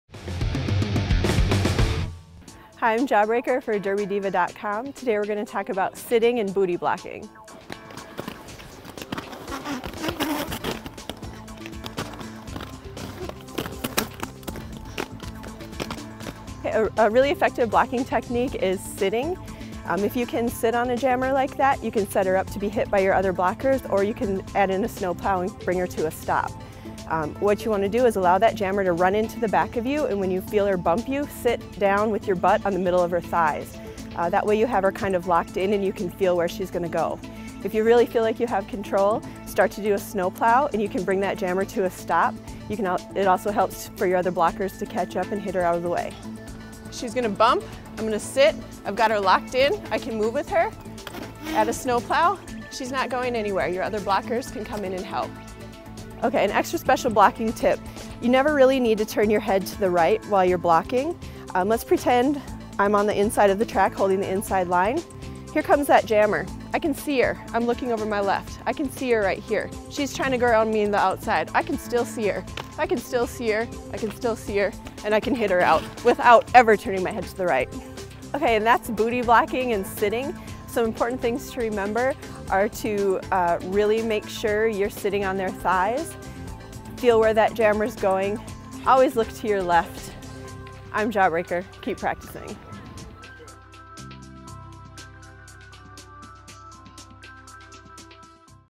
Roller derby: Sitting and Booty Blocking
How to block using the sitting technique. Explained by: Kim G "Jawbreaker" veteran skater from the North Star Roller Girls and Team Awesome.

a roller derby supply store based in New York.

"Win Some, Bruise Some."